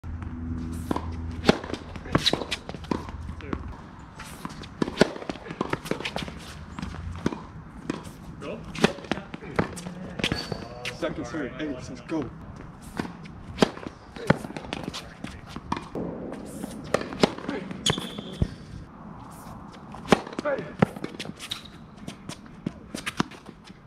USTA 3.5 serves BOMBS!
Welcome Trey from @WINNER@Winners-Only to the channel! Trey has a tennis YouTube channel too documenting his progress in the sport as he recently picked up tennis. He's developed a big serve pretty quickly and plays at the USTA 3.5-4.0 level. Check out his channel:

Leave some nice comments below for me and Trey for playing this match on The Challenge Court!

USTA Ratings:
Winston - USTA 4.5
Trey - USTA 3.5

External Battery